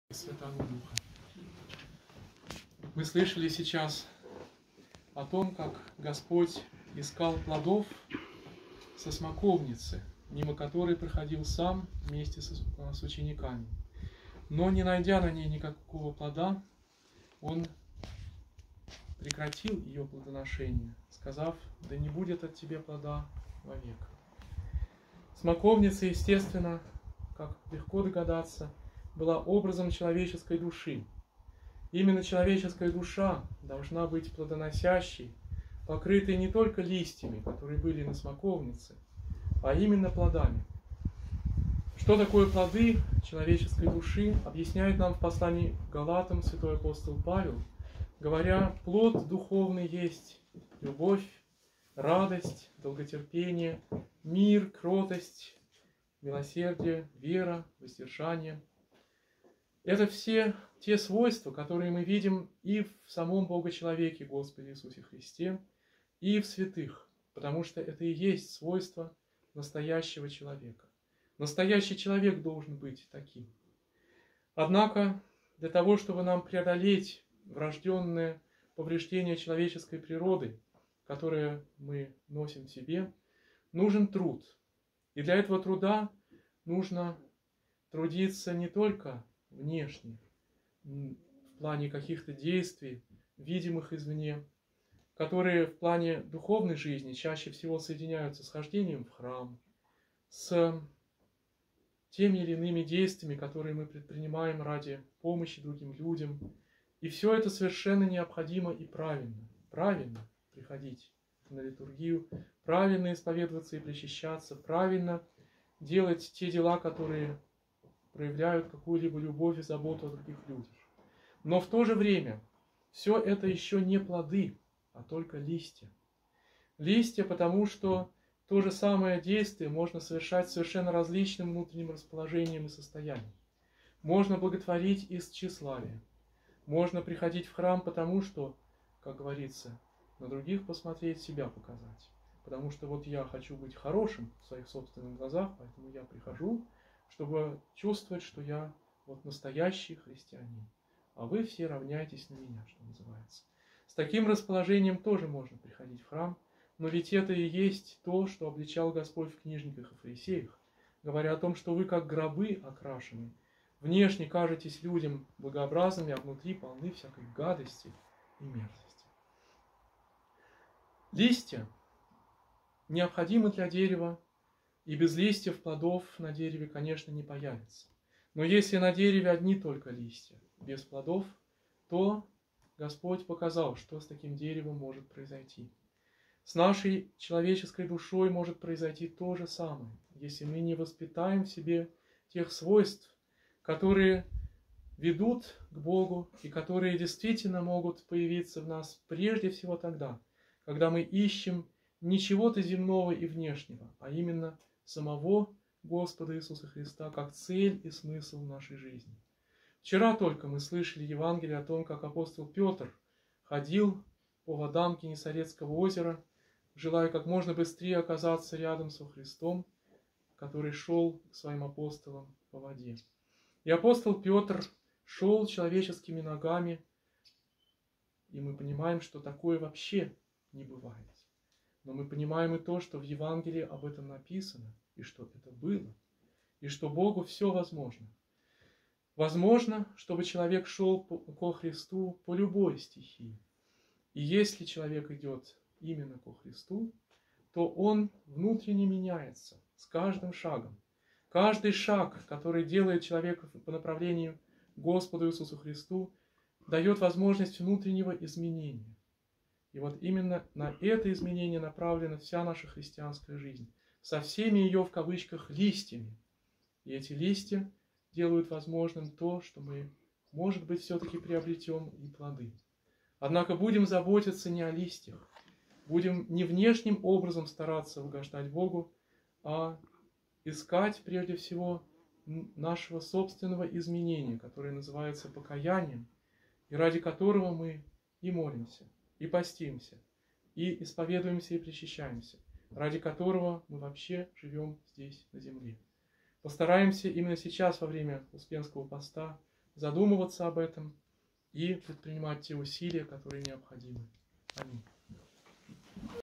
"Плоды, а не одни листья". Мф. 21, 18-22. Проповедь прот. Димитрия Свистова 15.08.22
Скит всех русских святых в Мурмелоне (Франция)